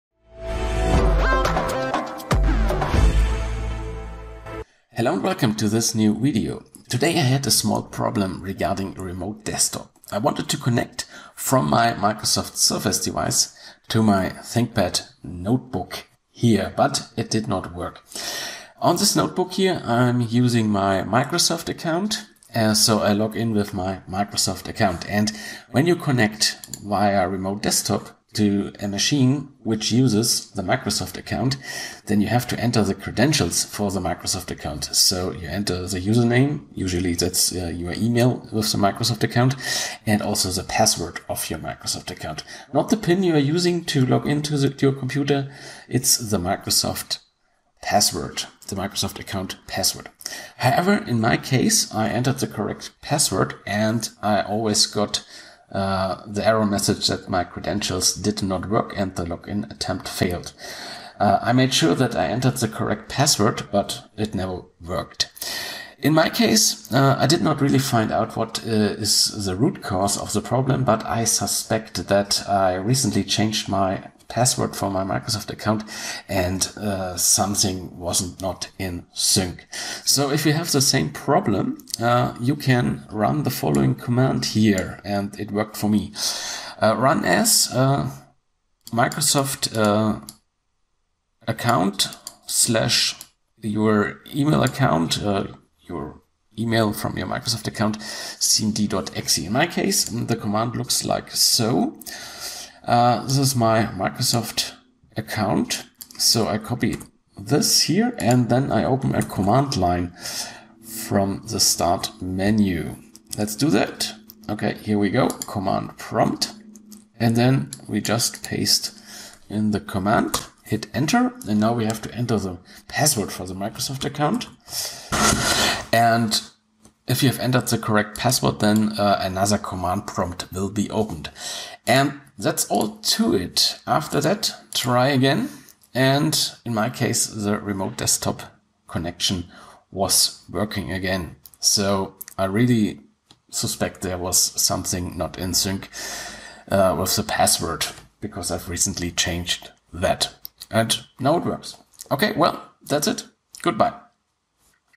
Remote Desktop: Error when logging in with Microsoft account
Problems connecting Remote Desktop, when using a Microsoft account.
Today I wanted to connect from my Microsoft Surface to my Thinkpad, using RDP. Unfortunately, the login did not work, I only got the message that the connection cannot be established.
Your credentials did not work. The logon attempt failed.